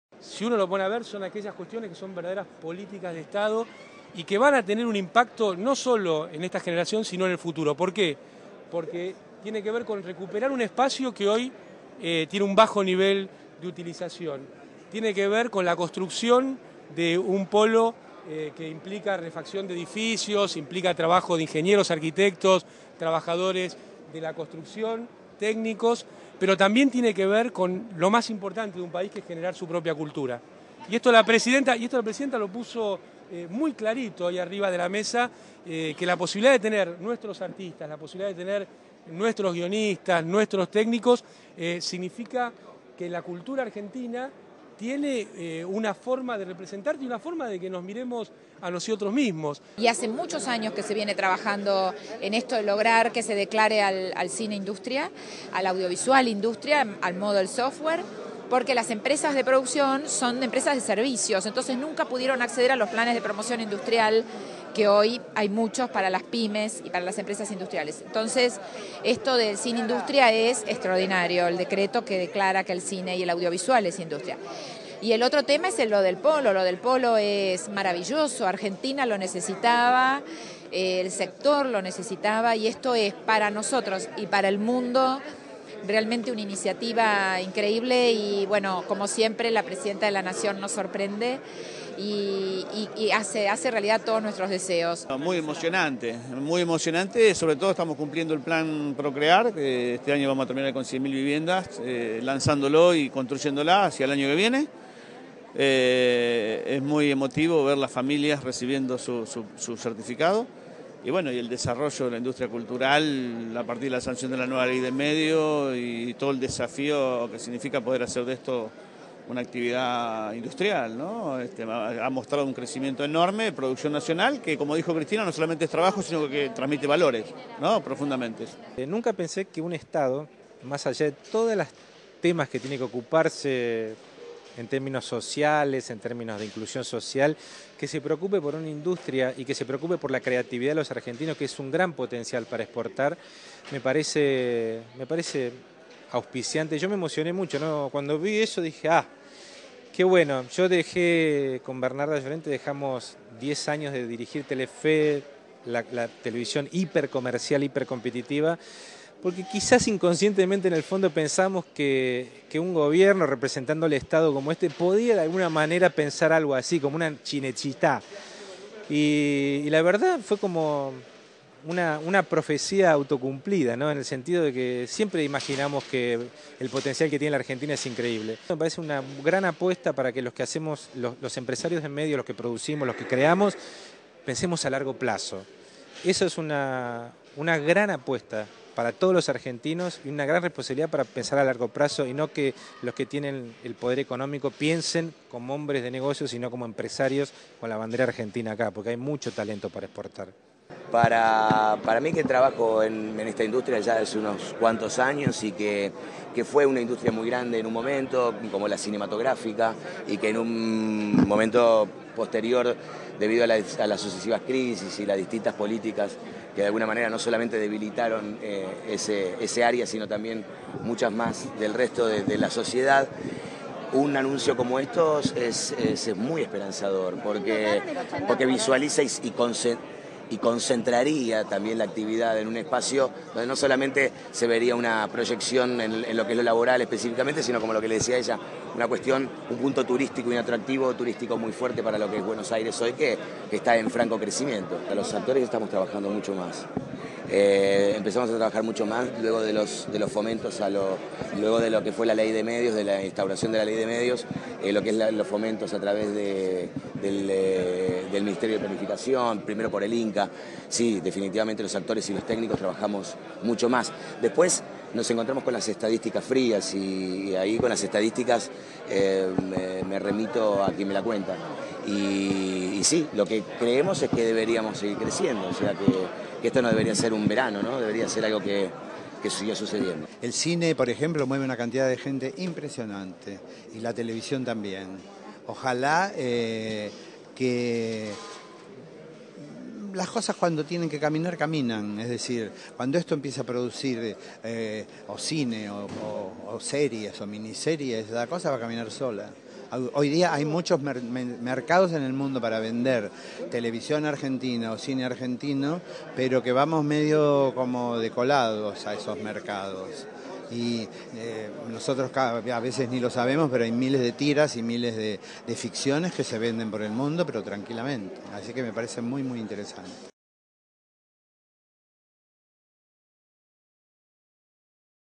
Boudou: "el polo tendrá impacto no solo en esta generación, sino en el futuro"
El vicepresidente Amado Boudou sostuvo hoy que los anuncios realizados por la presidenta Cristina Fernández de Kirchner para la industria audiovisual tendrán "impacto no sólo en esta generación, sino el futuro", ya que "posibilitará lo más importante para un país, que es generar su propia cultura".
    "Si uno se pone a ver son cuestiones que tienen que ver con verdaderas políticas de Estado, y que van a tener un impacto no sólo en esta generación, sino también en el futuro", dijo Boudou tras los anuncios realizados por la Presidenta en un acto efectuado en el Museo del Bicentenario de la Casa Rosada.
    Boudou enfatizó la importancia de la creación del Polo de Desarrollo de la Industria Audiovisual en la isla Demarchi, lo cual "si uno se pone a ver son cuestiones que tienen que ver con verdaderas políticas de Estado y que tendrán un impacto no sólo en esta generación sino también en el futuro".
    Consideró además que de este modo "se recupera un espacio que hoy tiene un bajo nivel de utilización, y con la construcción de un ámbito que implica la reconstrucción de edificios, el trabajo de ingenieros, arquitectos, técnicos, trabajadores de la construcción".
    "Pero también tiene que ver con lo más importante de un país, que es generar su propia cultura", destacó Boudou.
    "Y esto hoy la Presidenta lo puso muy claro sobre la mesa: es la posibilidad de tener nuestros artistas, nuestros guionistas, nuestros técnicos, significa que la cultura argentina tiene una forma de representarnos y que nos miremos nosotros mismos, porque los pueblos muchas veces tienden a desvalorizarse y asumir culturas que no son propias", aseveró.
    Dijo que además Cristina "fue muy clara con lo que tiene que ver con la participación de todos", al incluir la representación de la Ciudad de Buenos Aires en el Comité de Selección de proyectos.
    "Ella siempre habla de los 40 millones, habla de un país para todos, incluso para los que piensan distinto. En este caso informó que en el Comité de Selección de proyectos estarán la Anses, la Secretaría de Obras Públicas, el nuevo ente de administración de bienes del Estado y además el gobierno porteño".
    Por su parte, la Presidenta del Instituto Nacional de Cine y Artes Audiovisuales, Liliana Mazure, destacó que "hace muchos años que se venía trabajando en esto de lograr al audiovisual industria porque las empresas nunca pudieron acceder a los planes de promoción industrial que hoy hay muchos".
El diputado nacional Edgardo Depetri resaltó la entrega de certificados de los planes Procrear y el desarrollo de la industria cultural, ya que "la producción nacional no solo genera trabajo sino que también transmite valores".
El productor televisivo Claudio Villarruel admitió que nunca pensó "que un Estado se fuese preocupar por esta industria y por la creatividad de los argentinos".
Además hizo una autocritica sobre el papel que juegan los empresarios de los medios y opinó que "ésta una gran apuesta para que los empresarios de medios pensemos a largo plazo y no que los que tienen el poder económico piensen como hombre de negocios sino como empresarios con la bandera argentina puesta".
El actor Pablo Echarri consideró  que "un anuncio como este es muy esperanzador porque generaría una proyección no solo en lo laboral sino también en lo turístico". Asimismo resaltó que "los actores estamos trabajando mucho más luego de lo que fue la ley de medios".
El actor Antonio Gasalla se refirió a la posibilidad de generar más empleo con y explicó que "el cine y la televisión mueven una cantidad de gente impresionante y cuando esto empiece a producir cine o series la actividad va a caminar sola".